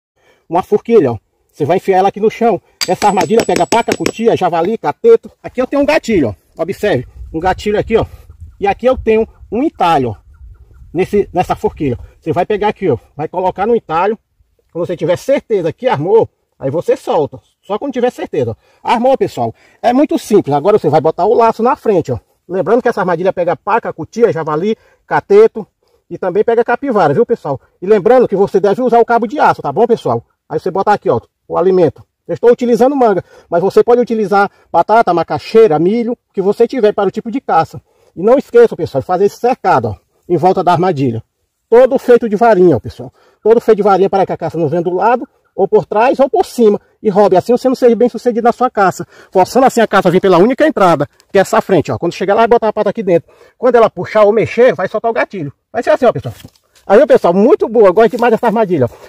Armadilha para pegar caças
Essa é uma armadilha de sobrevivência para pegar animais javali paca capivara tatu veado Cotia catitu preá mocó cateto porcão queixada anta quati tatupeba coelhos